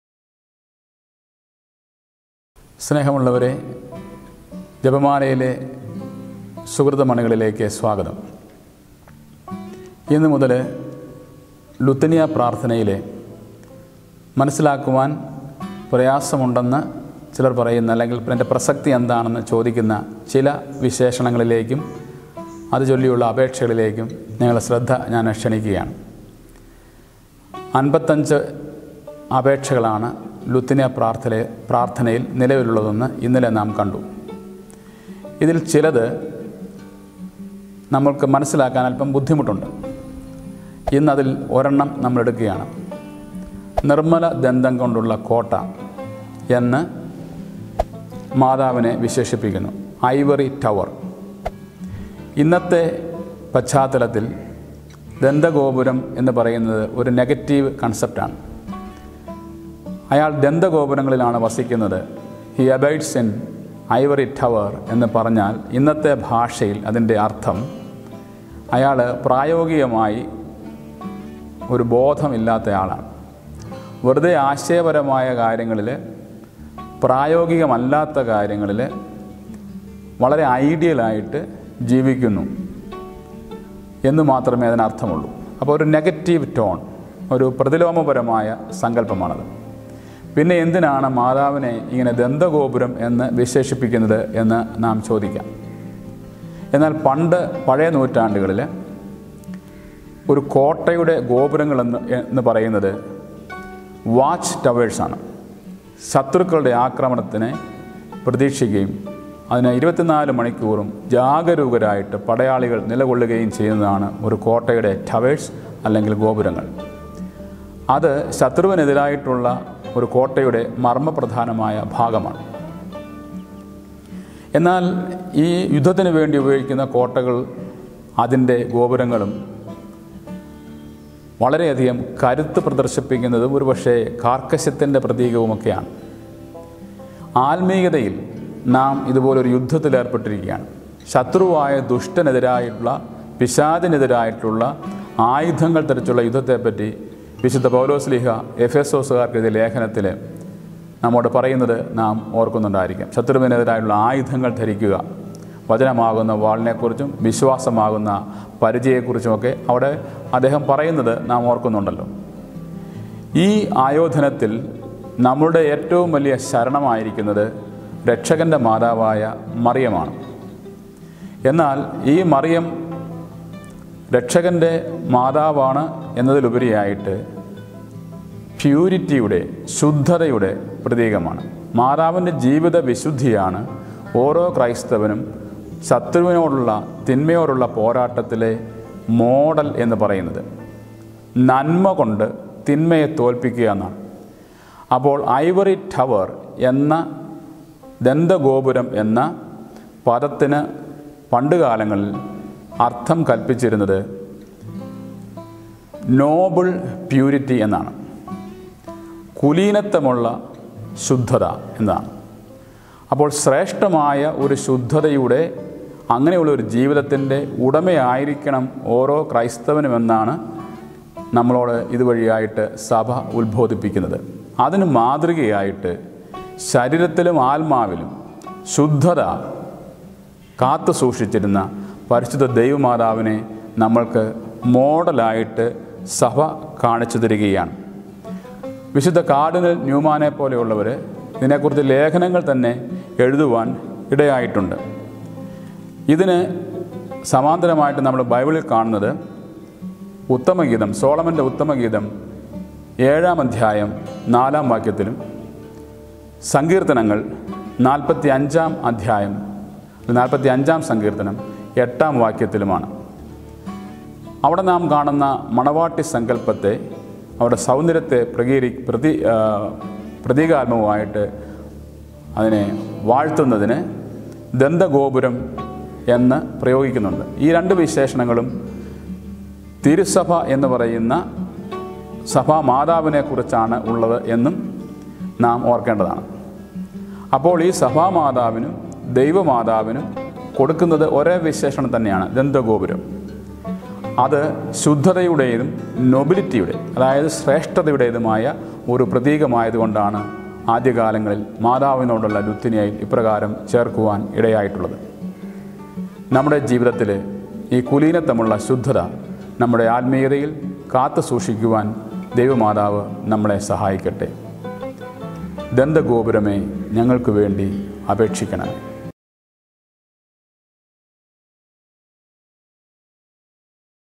"നിർമ്മലദന്തം കൊണ്ടുള്ള കോട്ട"|| Episode 24 || Briefing by Rev. Fr. Jose Mariadas OIC
"നിർമ്മലദന്തം കൊണ്ടുള്ള കോട്ട" എന്ന് മാതാവിനെ വിശേഷിപ്പിക്കാൻ കാരണം എന്ത് ? || Episode 24 || Briefing by Rev. Fr. Jose Mariadas OIC